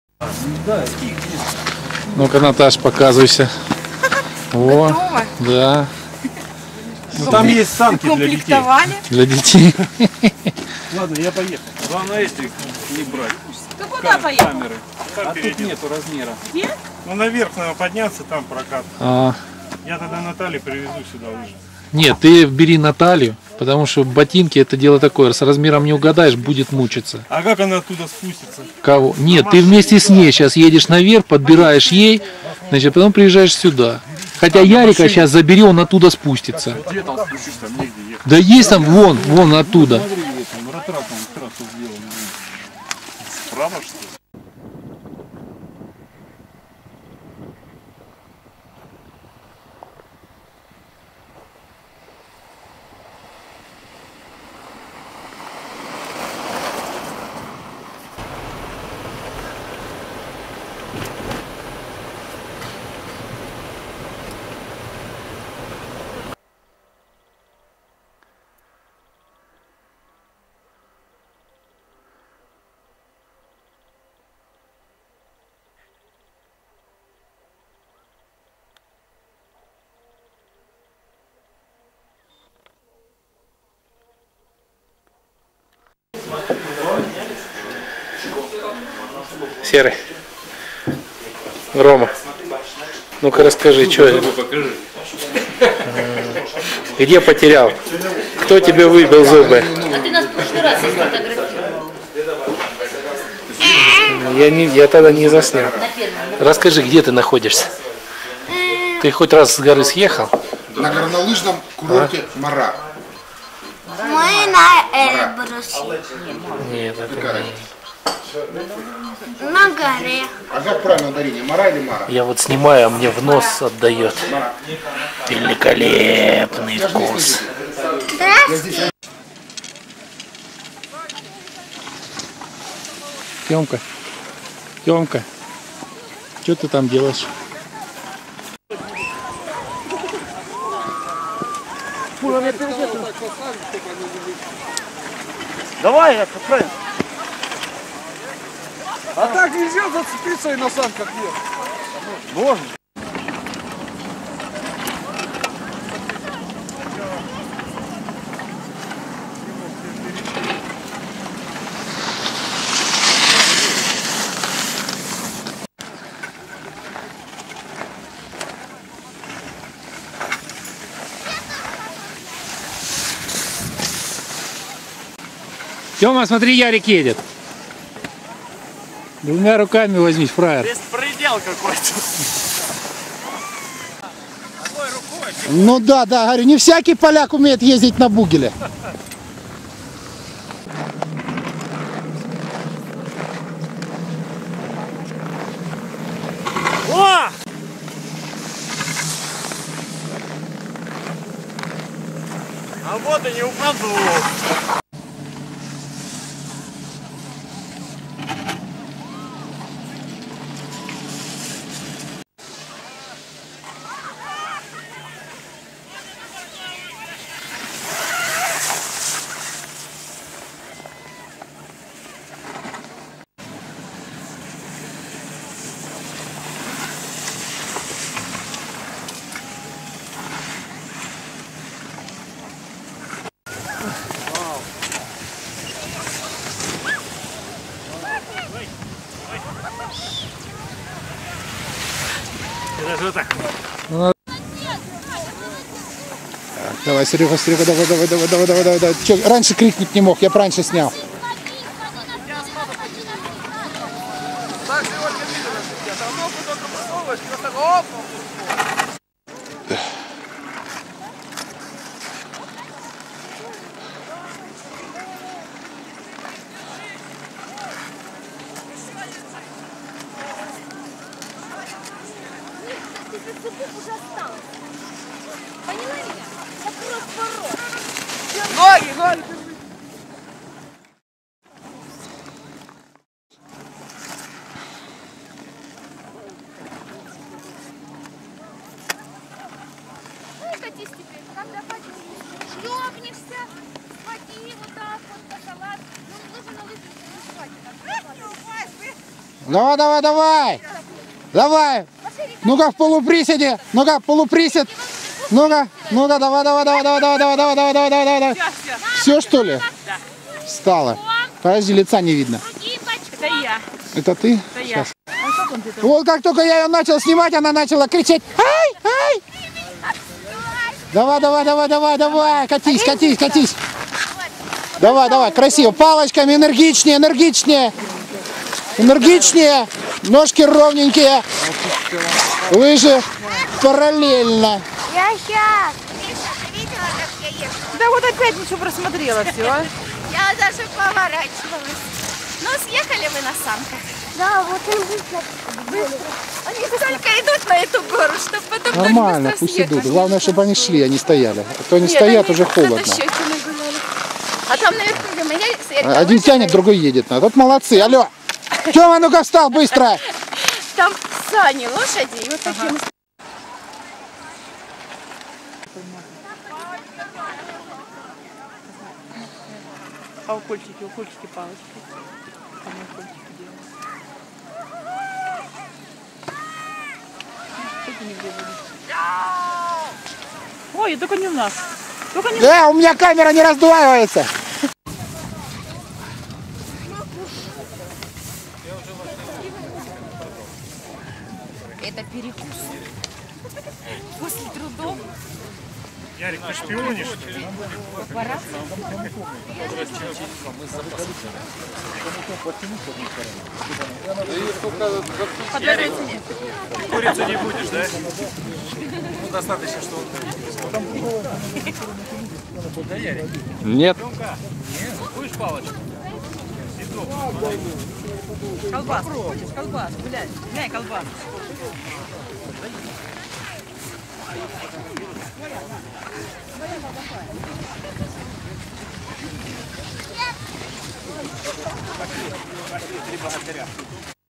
2012 Мара Подготовка к Полонийной олимпиаде.
С большим энтузиазмом члены РООСК «Объединенные Польские Сердца», приняли участие в подготовительных соревнованиях к «X Всемирным Зимним Полонийным Играм»., которые состоятся с 25 февраля по 3 марта 2012г. в 5 городах Польши( Bielsku-Białej, Cieszynie, Szczyrku, Wiśle, i Tychach).